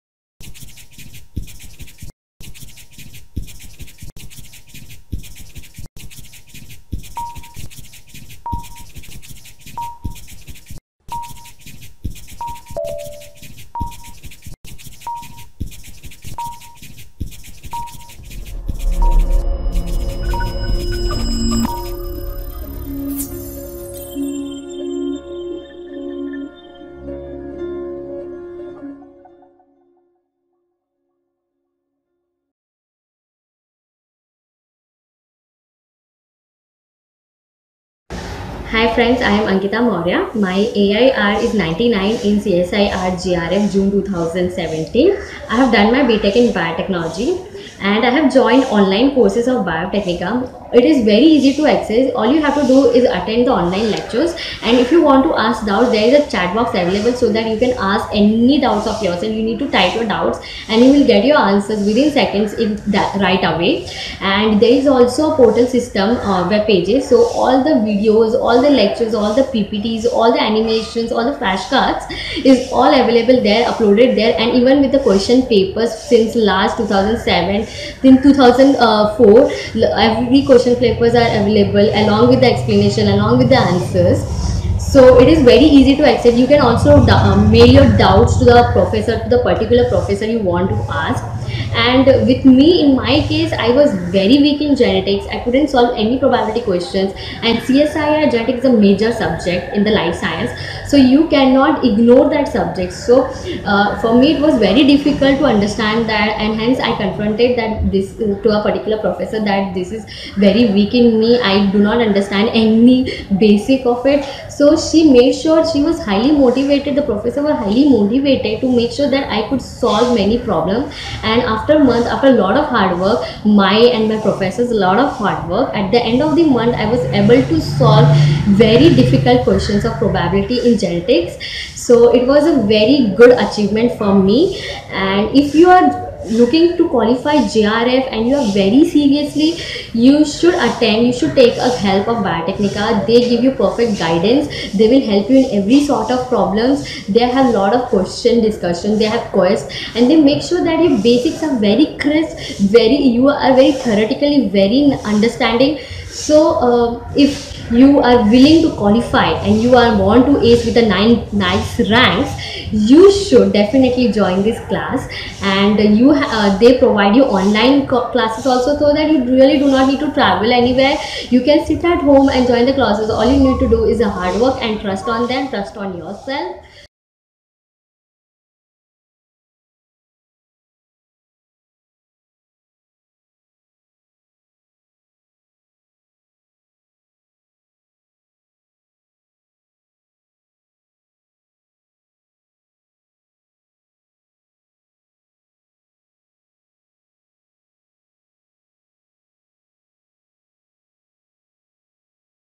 Ankita Maurya, CSIR NET Topper shares her Review of BioTecNika's Coaching
BioTecNika is the only coaching company to post verified feedback videos of its toppers, Watch out the Journey of Ankita Maurya - CSIR NET Topper with 99th Rank and how she got helped by BioTecNika's Teachers